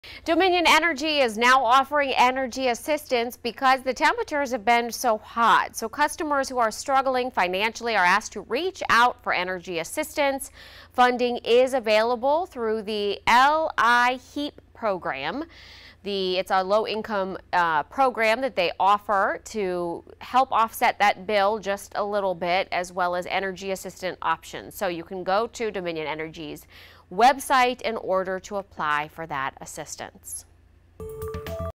Dominion Energy offers assistance
Customers who are struggling financially are asked to reach out for energy assistance funding.